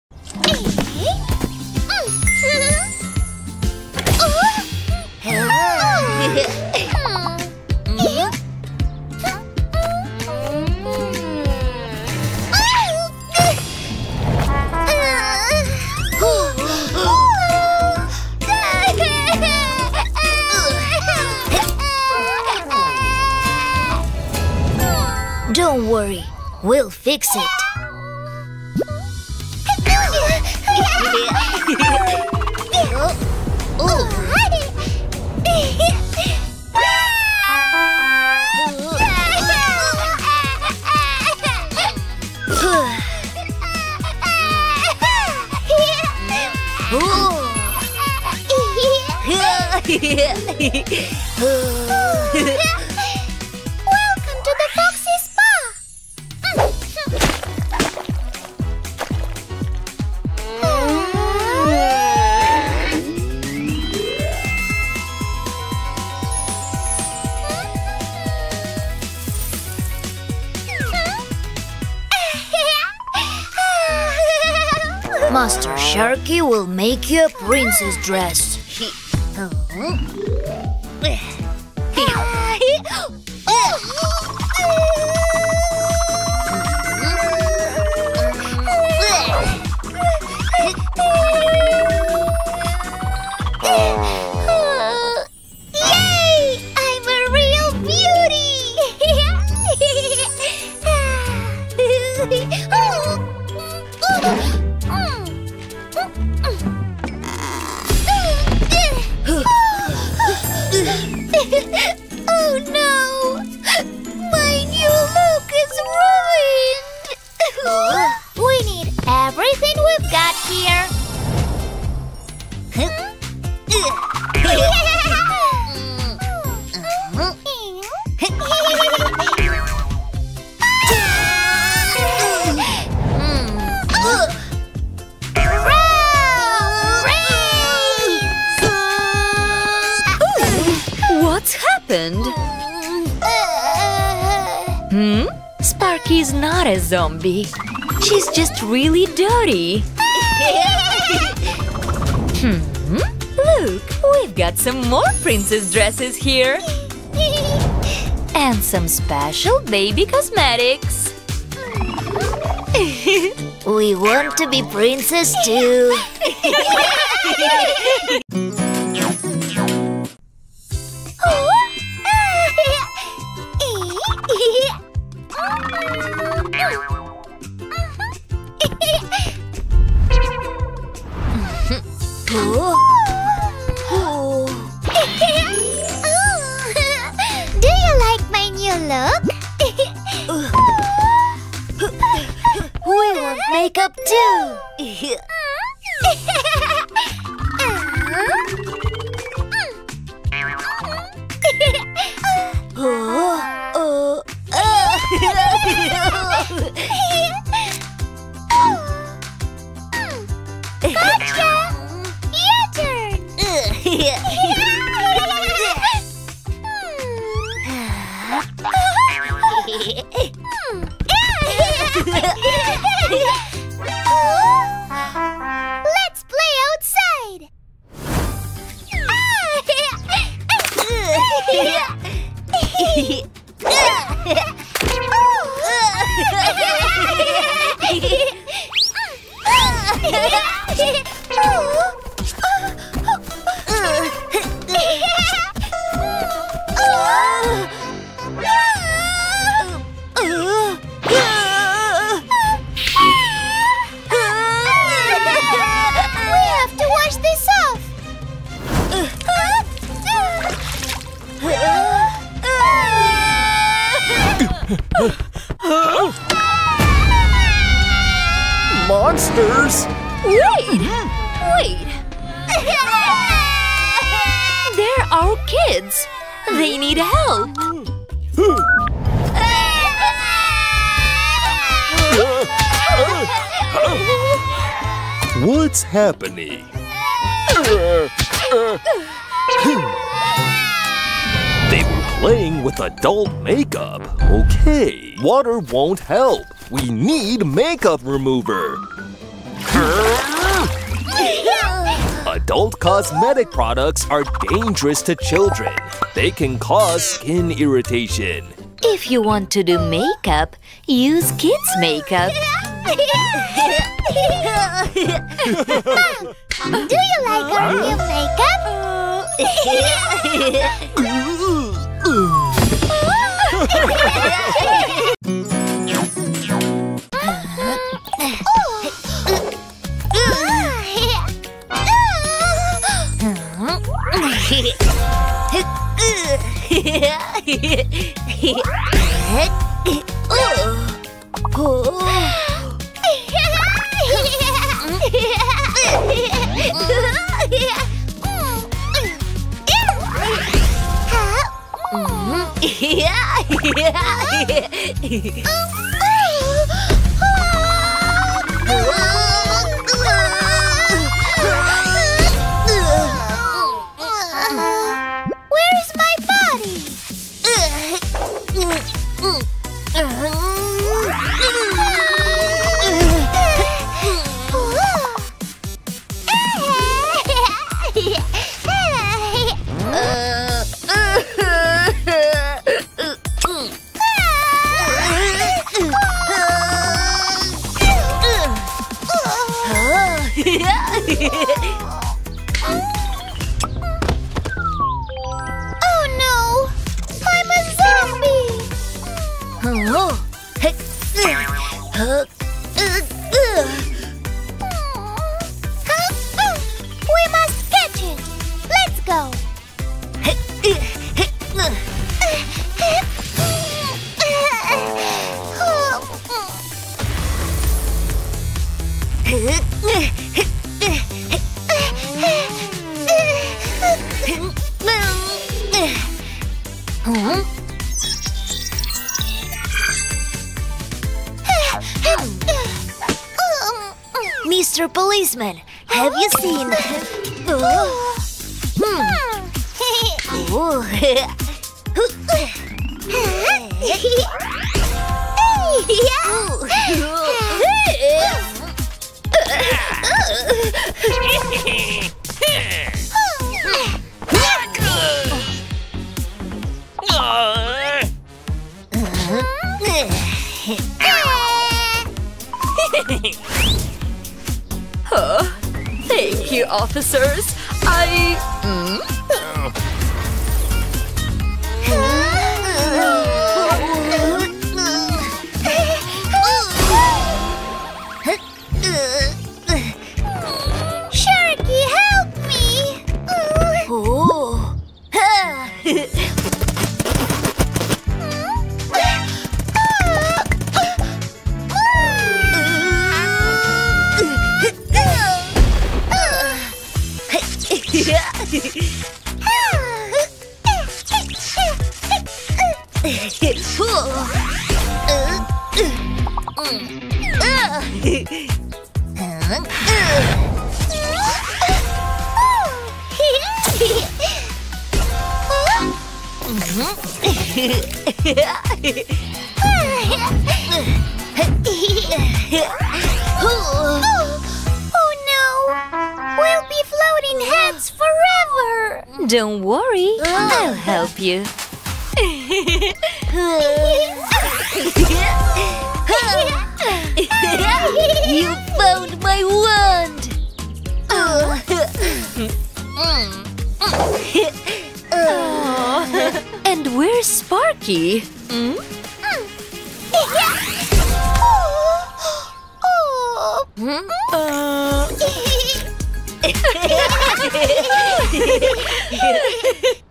Beauty Makeup for Princess 🎀👑 Funny Cartoons + Nursery Rhymes by Sharky&Sparky
00:00 PRINCESS DRESS
01:16 RAINBOW MAKEOVER
04:08 DON'T USE ADULT MAKEUP!
06:47 WHERE IS MY BODY?